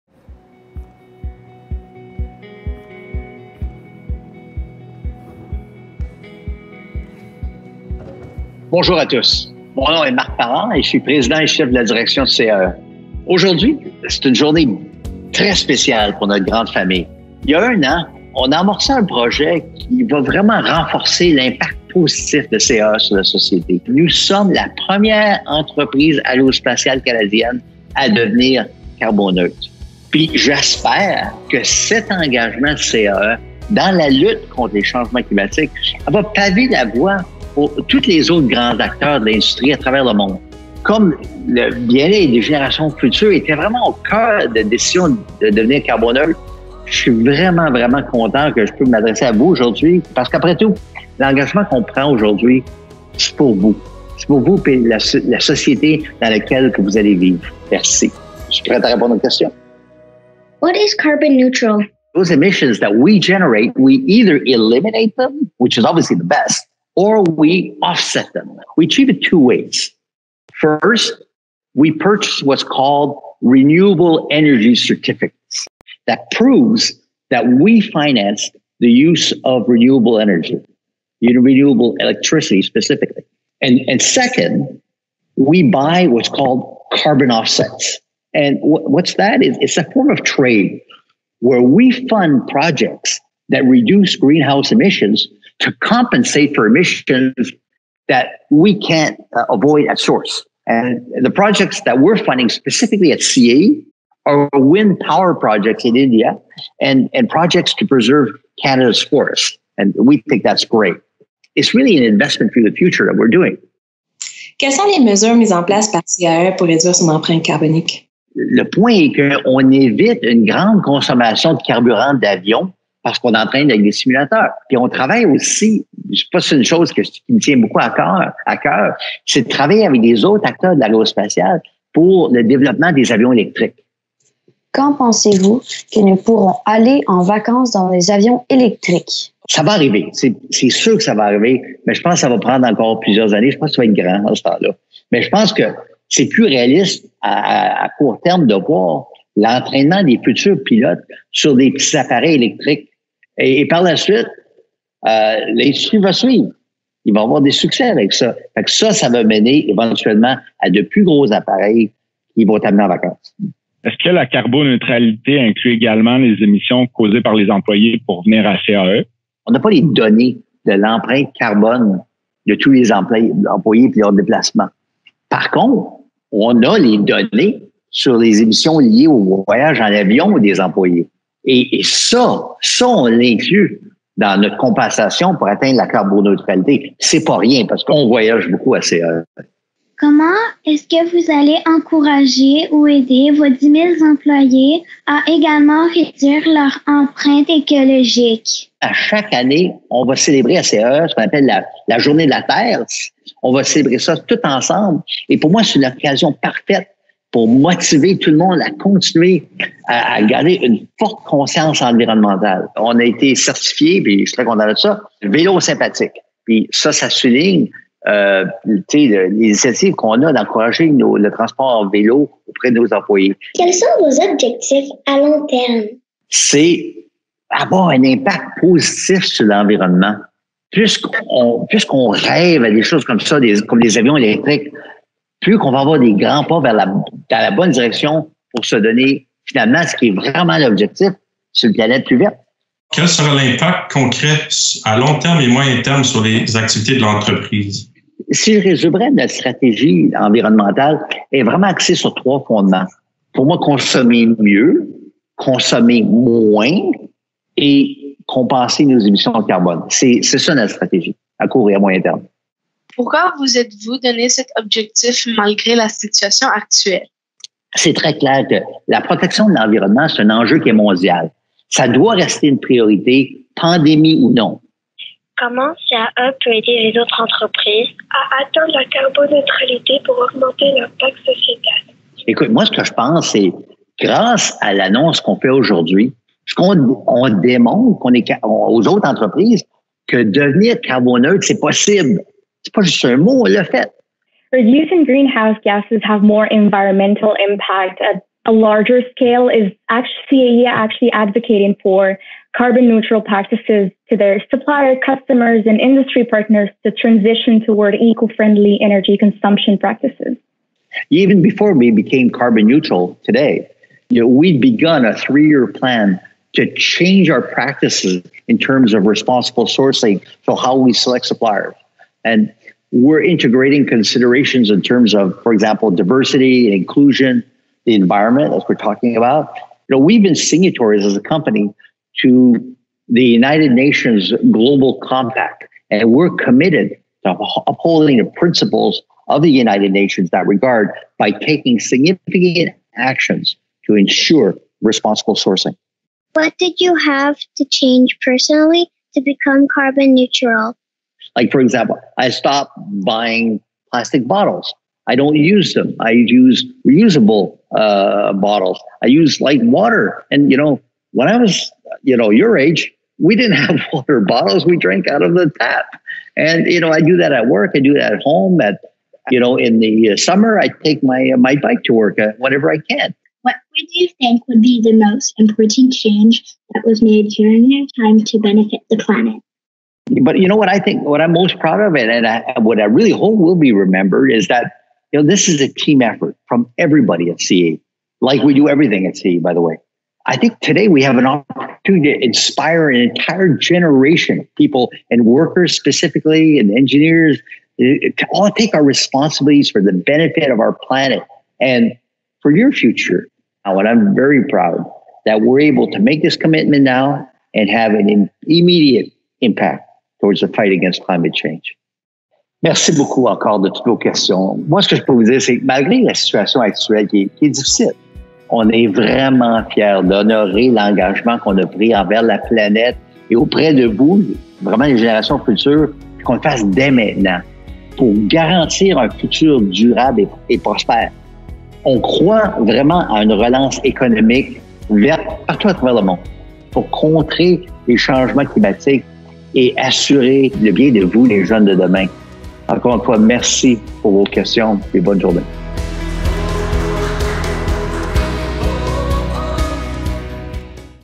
CAE's President & CEO on carbon neutrality / Le président de CAE parle de carboneutralité
Full Q&A / Toutes les questions et réponses

CAE becomes first Canadian aerospace company to become carbon neutral. CAE’s President and CEO sat down with employees and their children for a live virtual discussion on the environment and CAE’s commitment.
CAE est la première entreprise aéronautique canadienne à devenir carboneutre. Le président et chef de la direction de CAE a entamé une discussion virtuelle en direct avec des employés et leurs enfants sur l’environnement et l’engagement de CAE.